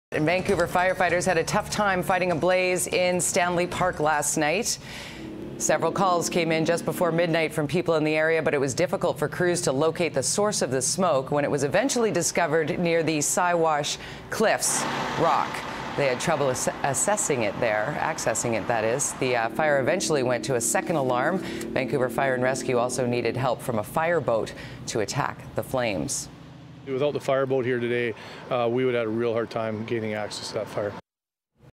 Fire in Stanley Park
Vancouver firefighters had a tough time fighting a blaze in Stanley Park last night near the cliffs of Siwash Rock.

Vancouver Fire and Rescue needed help from a fireboat.